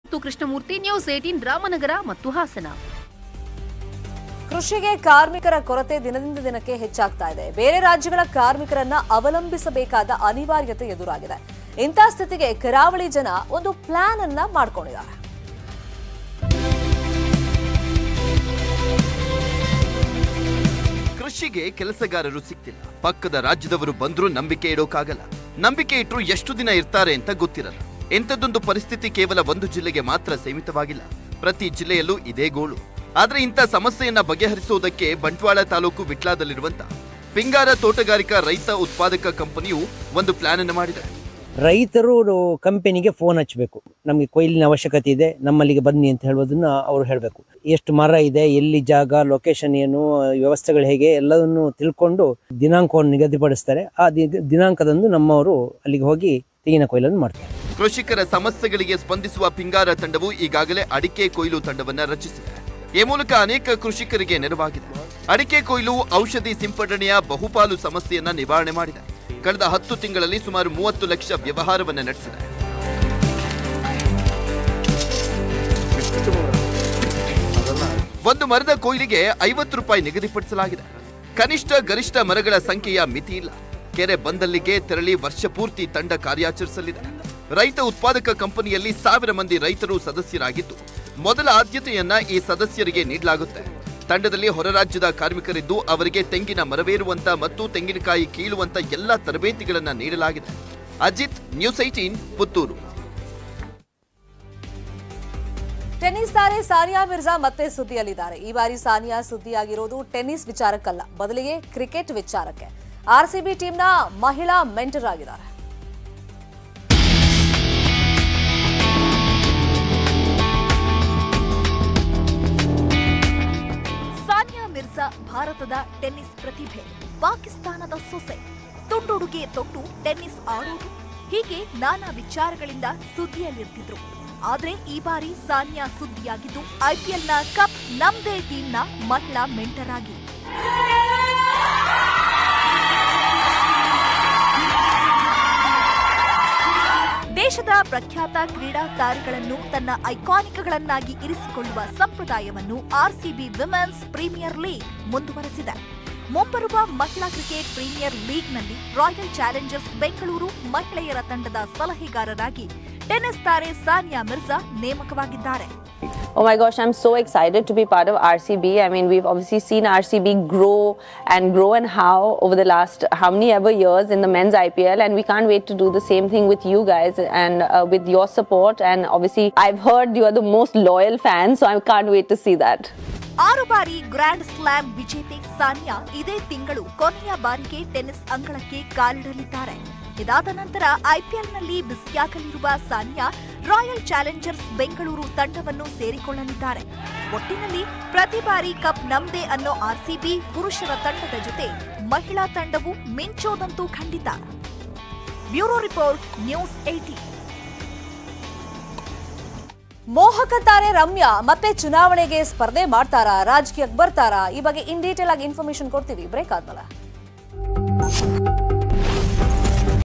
Puttur Krushi Karmika | ಬಂಟ್ವಾಳ ತಾಲೂಕಿನಲ್ಲಿ ಅಡಿಕೆ ಕೊಯ್ಲಿಗೆ ಪ್ರತ್ಯೇಕ ತಂಡ!
Puttur Krushi Karmika  | ಕೃಷಿಗೆ ಕೆಲಸಗಾರರು ಸಿಗ್ತಿಲ್ಲ. ಪಕ್ಕದ ರಾಜ್ಯದವರು ಬಂದ್ರೂ ನಂಬಿಕೆ ಇಡೋಕಾಗಲ್ಲ. ನಂಬಿಕೆ ಇಟ್ರು ಎಷ್ಟು ದಿನ ಇರ್ತಾರೆ ಅಂತ ಗೊತ್ತಿರಲ್ಲ. ಇಂಥದೊಂದು ಪರಿಸ್ಥಿತಿ ಕೇವಲ ಒಂದು ಜಿಲ್ಲೆಗೆ ಮಾತ್ರ ಸೀಮಿತವಾಗಿಲ್ಲ ಪ್ರತೀ ಜಿಲ್ಲೆಯಲ್ಲೂ ಇದೇ ಗೋಳು. ಆದ್ರೆ ಇಂಥಾ ಸಮಸ್ಯೆಯನ್ನ ಬಗೆಹರಿಸೋದಕ್ಕೆ ಬಂಟ್ವಾಳ ತಾಲೂಕು ವಿಟ್ಲದಲ್ಲಿರುವ ಪಿಂಗಾರ ತೋಟಗಾರಿಕಾ ರೈತ ಉತ್ಪಾದಕ ಕಂಪನಿಯು ಒಂದು ಪ್ಲಾನ್ ಮಾಡಿದೆ.